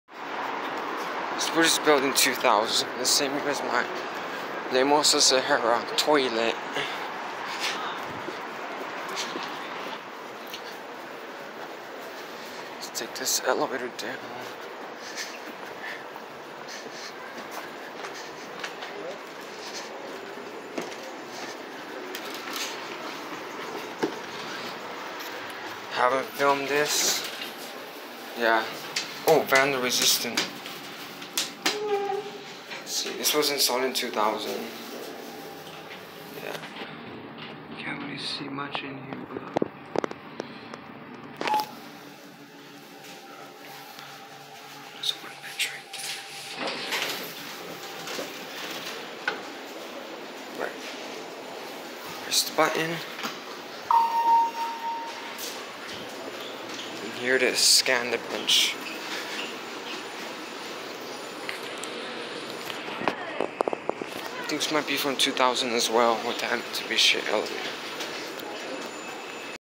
5155: 2000 Mitsubishi Hydraulic Elevator @ The Bridge of Gardens @ South Coast Plaza + wooden bench
This is my first time riding this elevator which reminds me of a subway elevator. This is my first time seeing these call stations. Notice how I pressed the call station  on the first floor and touched the bench.

Filmed: Friday December 9, 2022